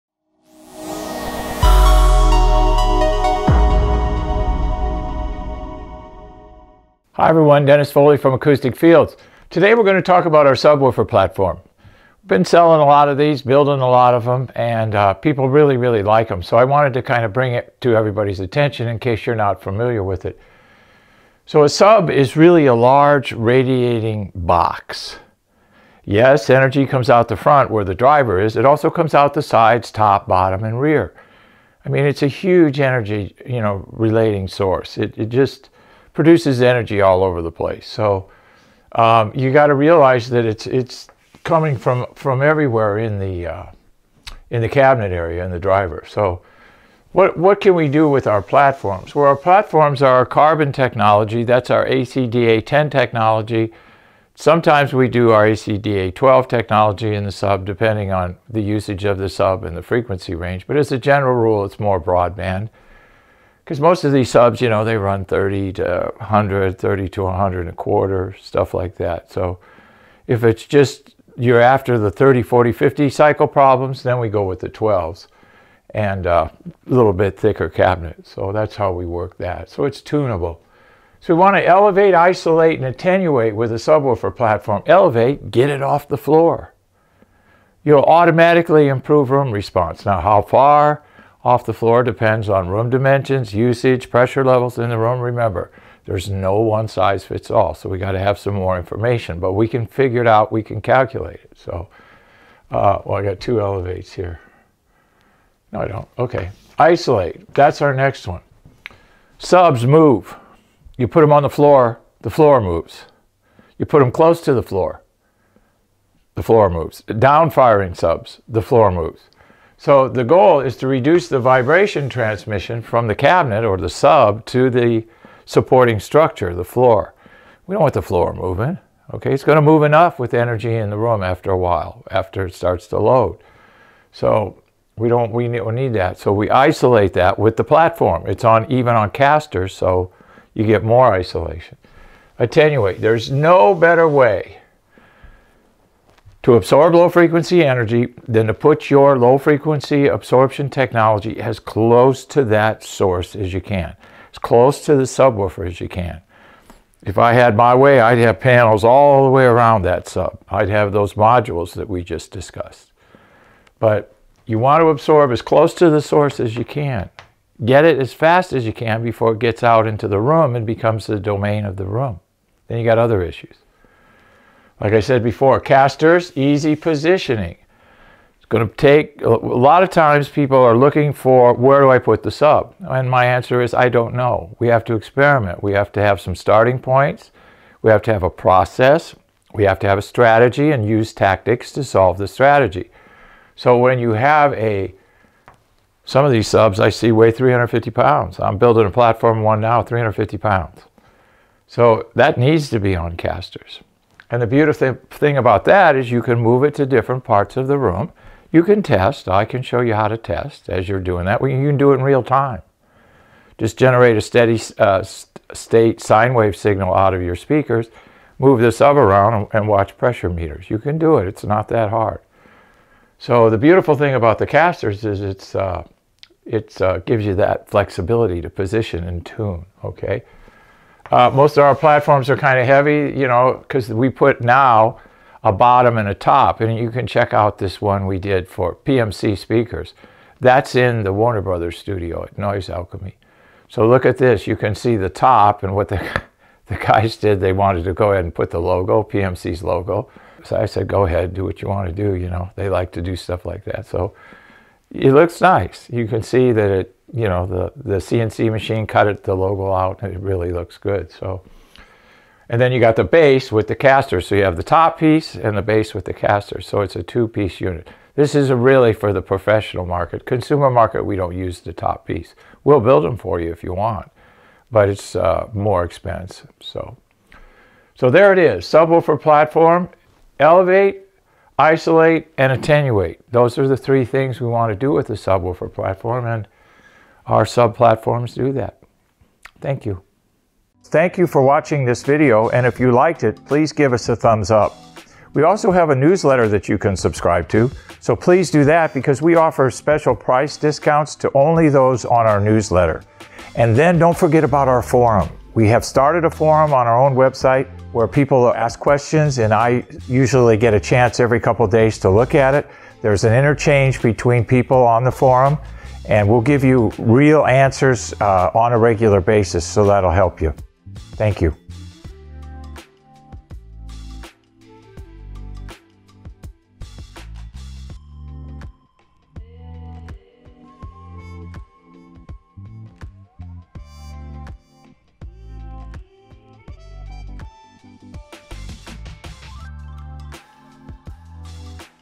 Subwoofer Platform
- - In this video we're going to talk about subwoofer platforms in our music rooms. Watch the video to find out more!